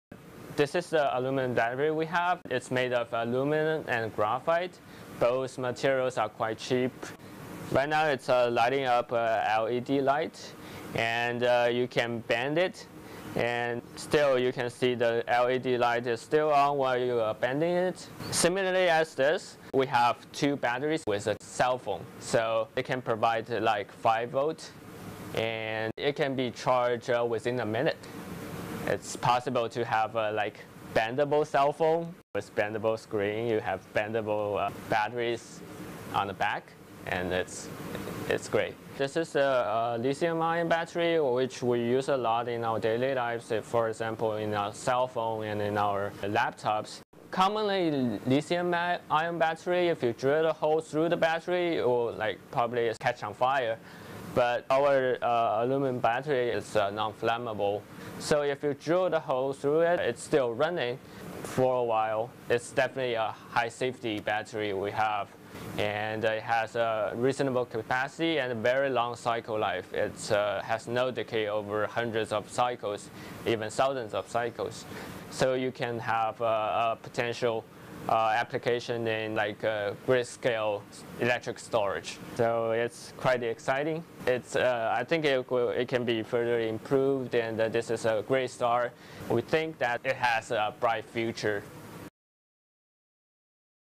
First look at the battery that can charge a phone in a minute
Scientists at Stanford University say they have invented a new prototype battery that can fully charge a smartphone in just one minute.
It is hoped the new aluminium battery will provide safer, more convenient, greener alternative for laptops and mobiles. Here, Stanford graduate student Ming Gong and postdoctoral scholar Yingpeng Wu demonstrate the new battery.